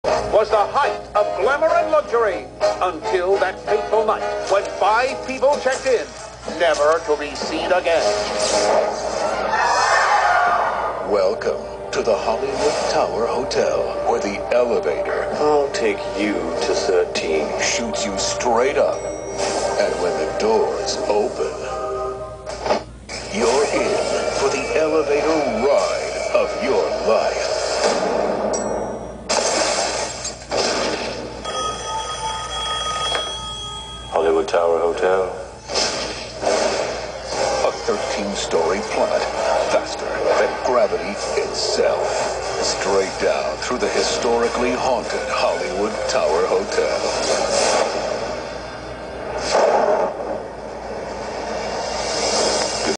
Disneyland Tower Of Terror Hotel
The Hollywood Tower was the height of luxury until one faithful night, 5 people checked in and was never to be seen again.